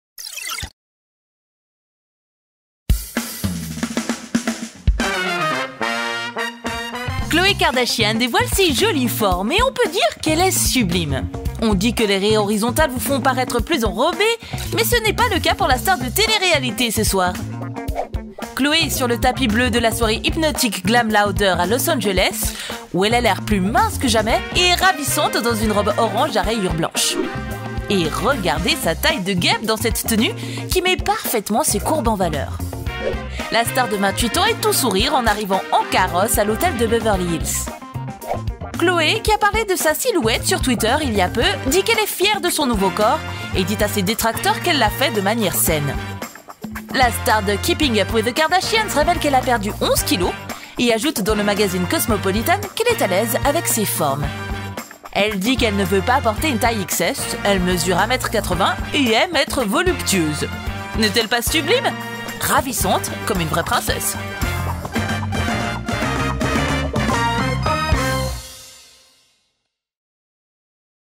Khloe Kardashian plus mince que jamais dans une robe à rayures
Khloe Kardashian dit être fière de son nouveau corps dans cette robe à rayures qui met parfaitement ses formes en valeur.